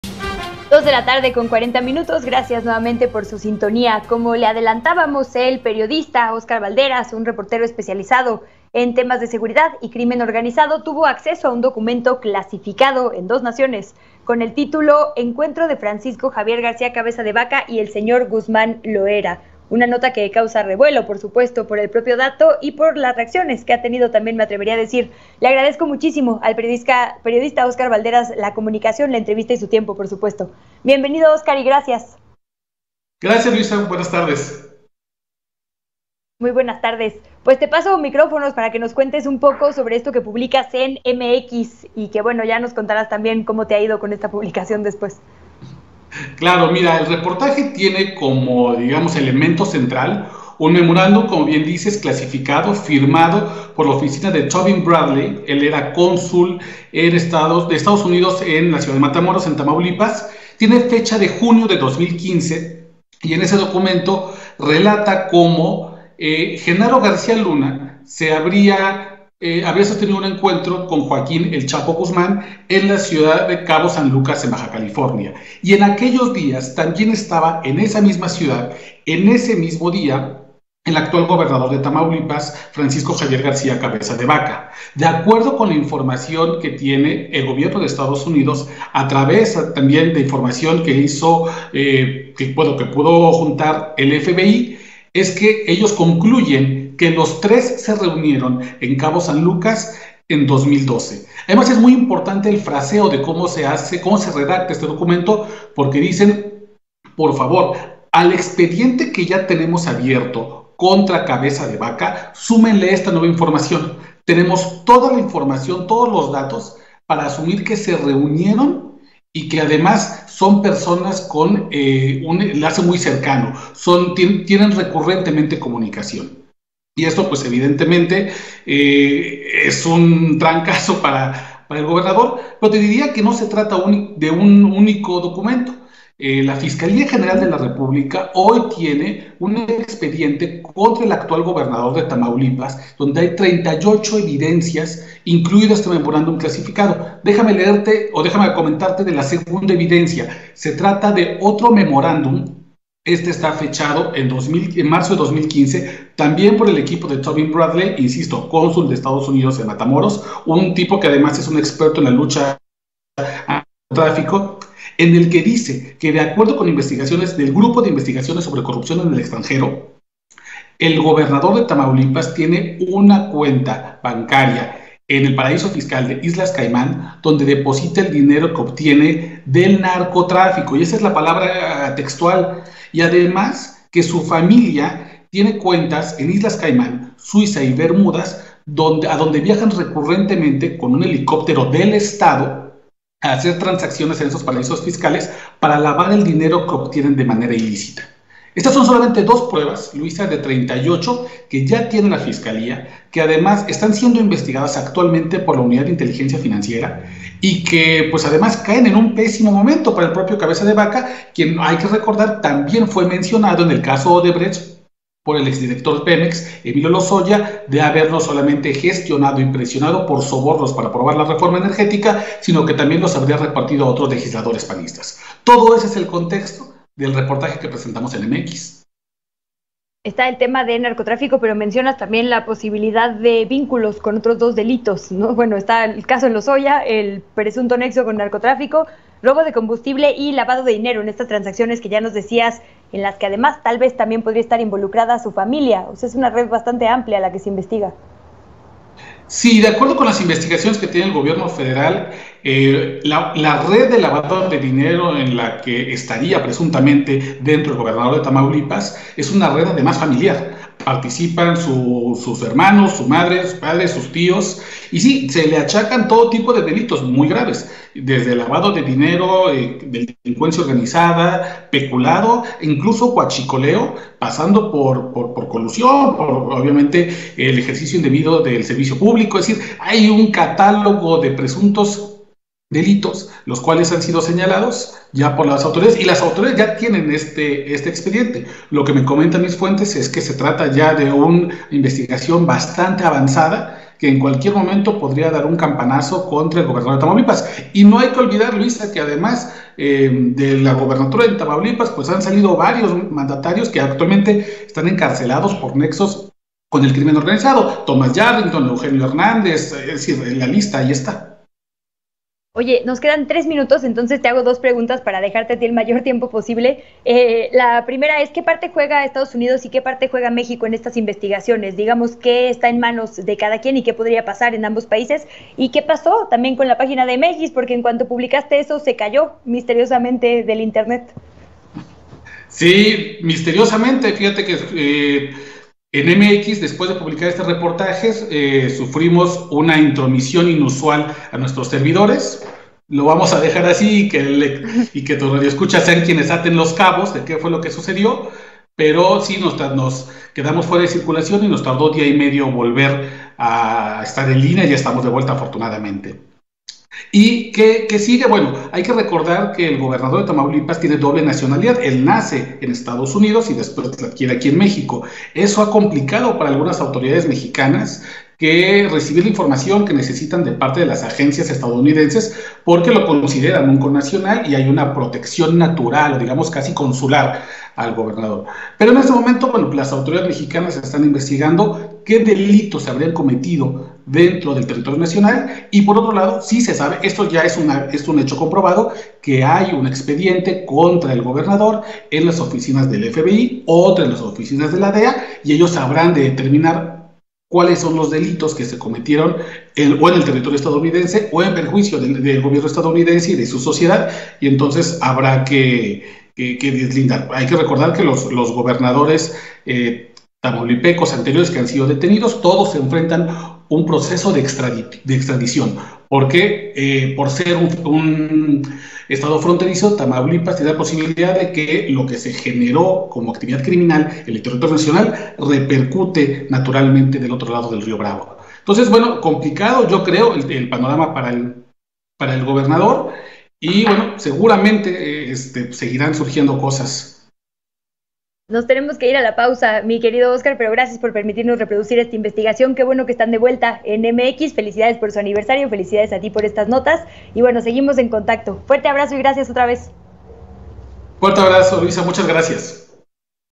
CABEZA DE VACA y su FAMILIA LAVAN DINERO del NARCO en ISLAS CAIMÁN: Balderas
El periodista Óscar Balderas menciona que de acuerdo con un memorándum de la oficina del diplomático Tobin Bradley, se concluyó que en 2012, García Luna tuvo un encuentro con 'El Chapo Guzmán'  y Francisco Cabeza de Vaca en Baja California.